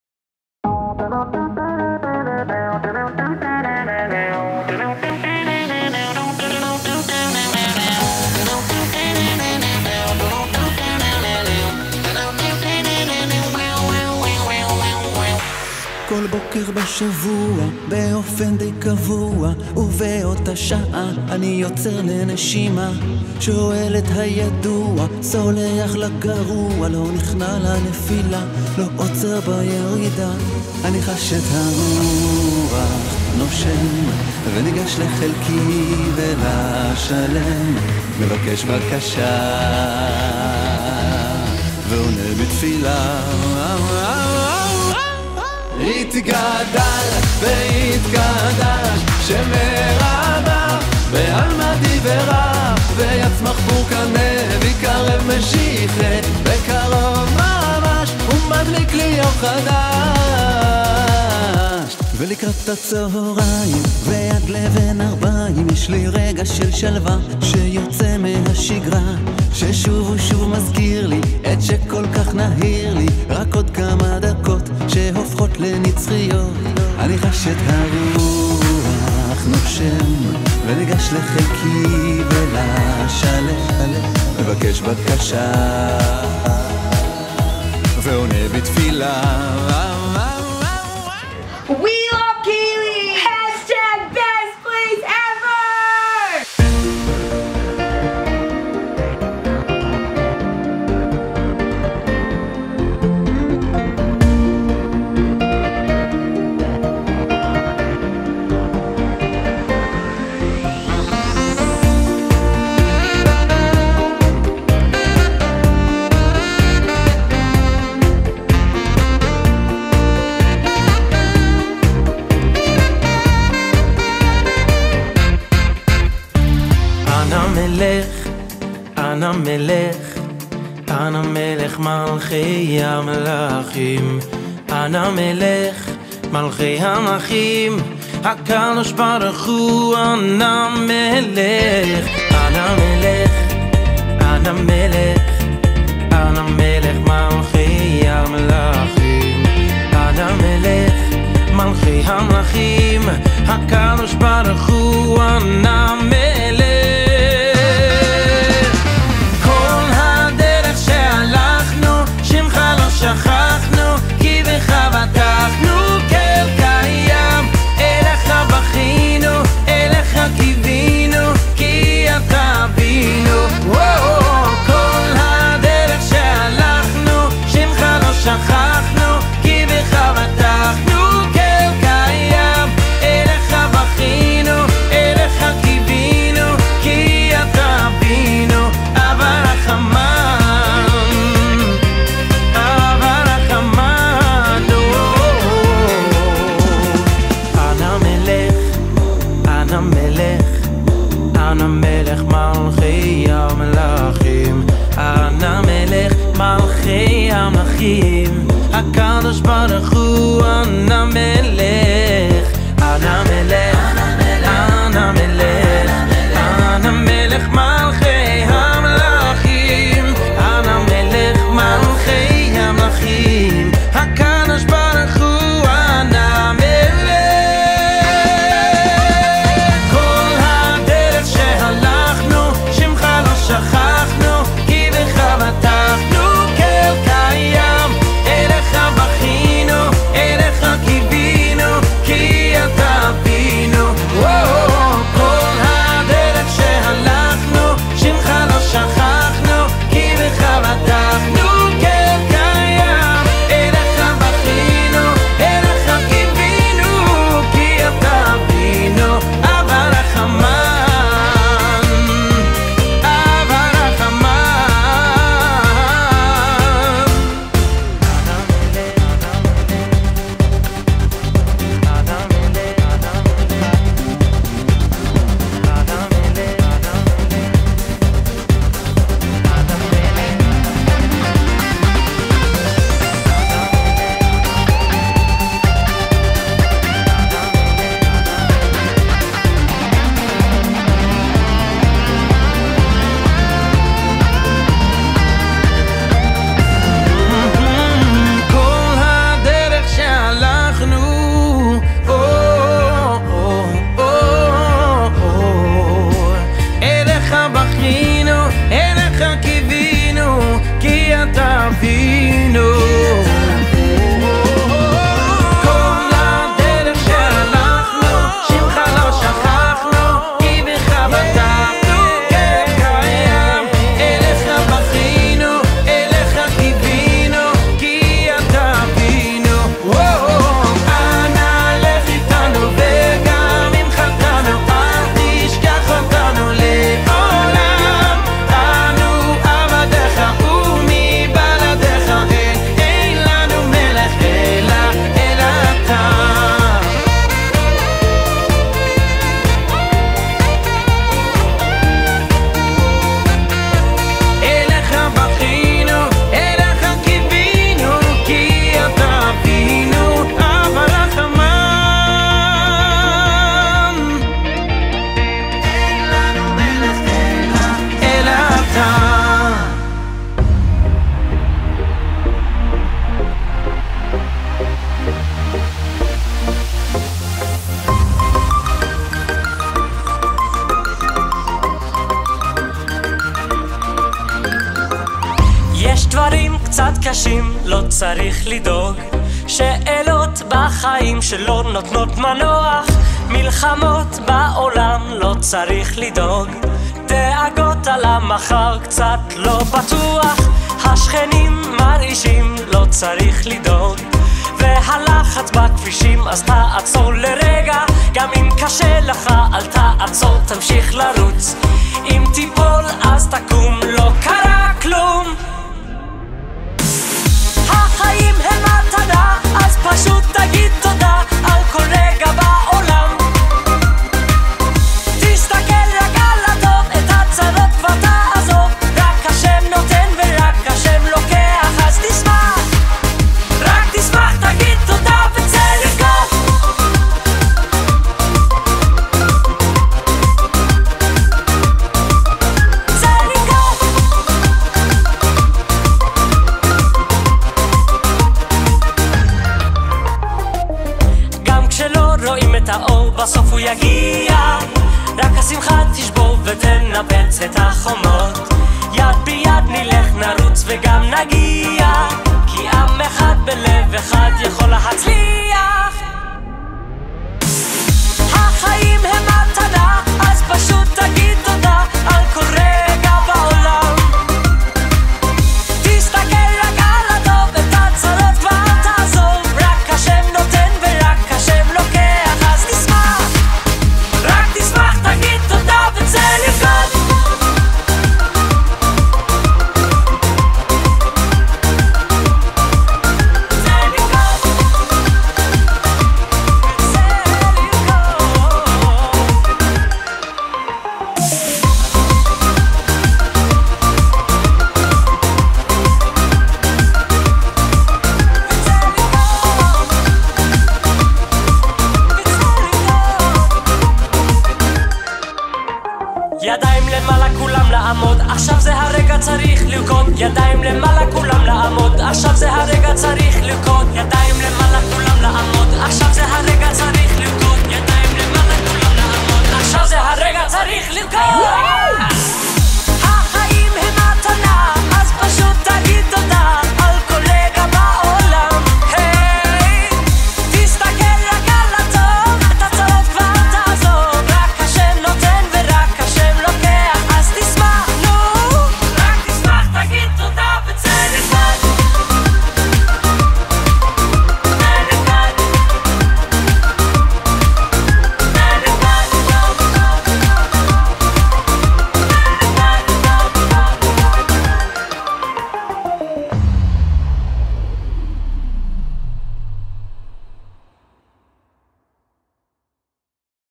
Kaylie Girls 2017: Staff Appreciation Video
Thank you for the most amazing summer ever! It was all because of you!!